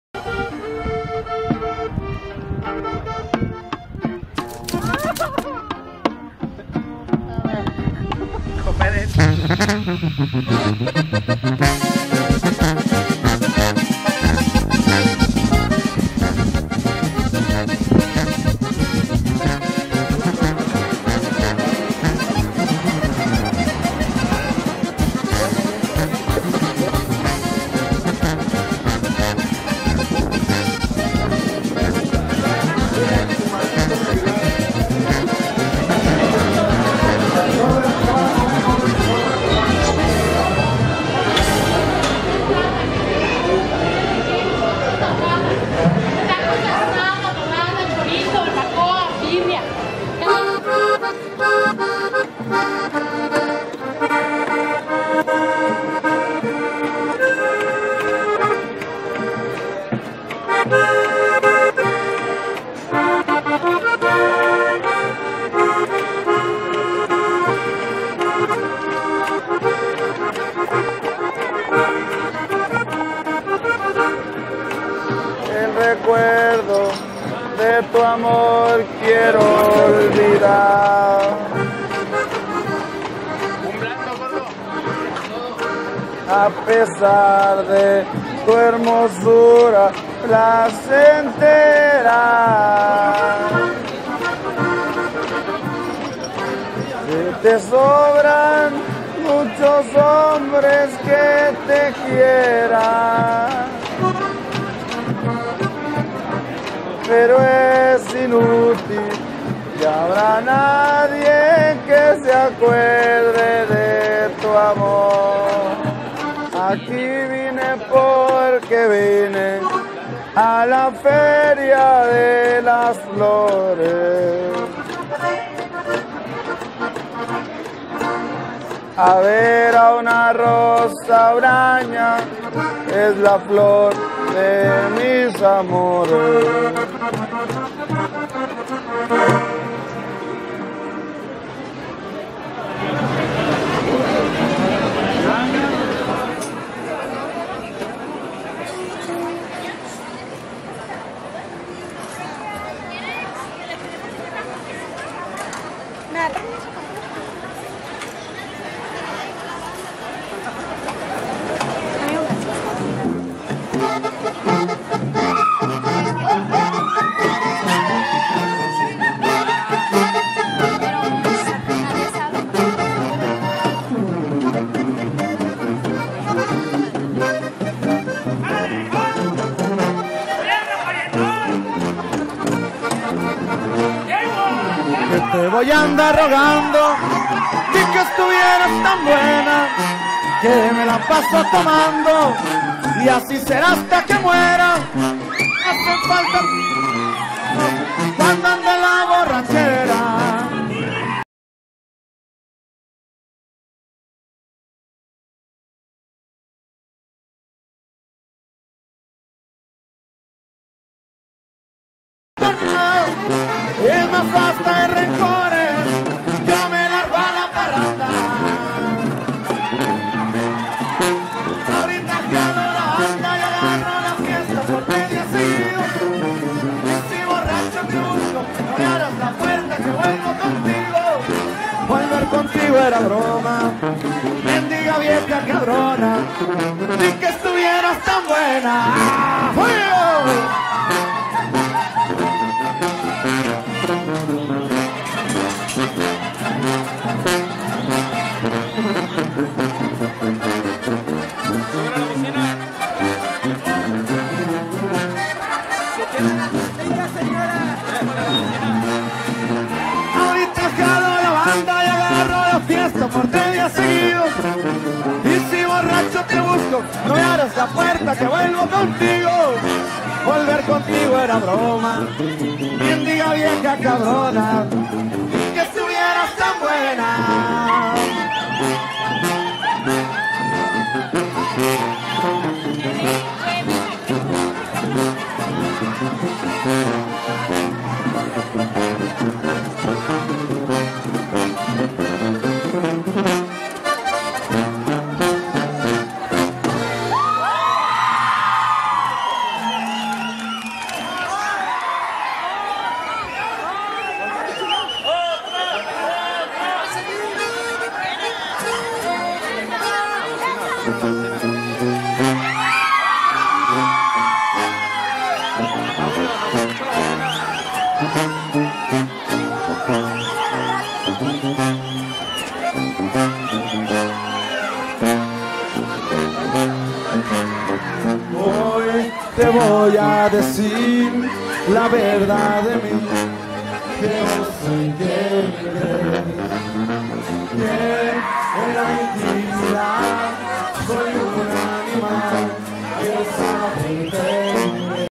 se hacen pasar por cantantes callejeros CALIBRE50 en sj de dios guad
increible actuacion de calibre 50 en el mercado de san juan de dios en guadalajara jalisco temazo